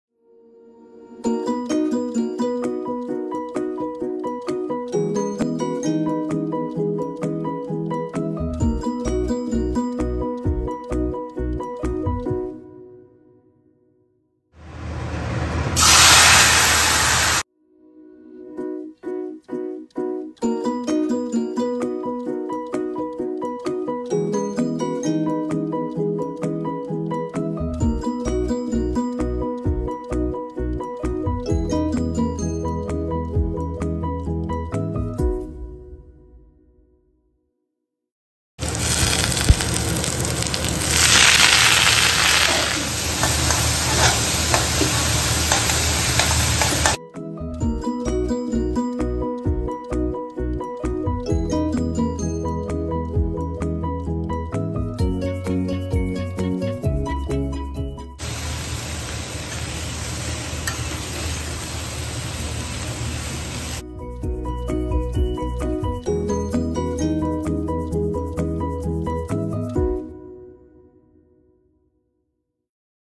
Assamese Dish (Fried Fish With Fermented Bamboo Shoot)
This is an age old Assamese recipe I want to share with you all. I am using a locally available fresh river fish (small size) You can try with any other small fish. Also if you don’t have the (KHARISA) the fermented bamboo shoot, you can use Sauerkraut (fermented cabbage). Its available in the US. Hope you all will love the recipe and try it out!❤️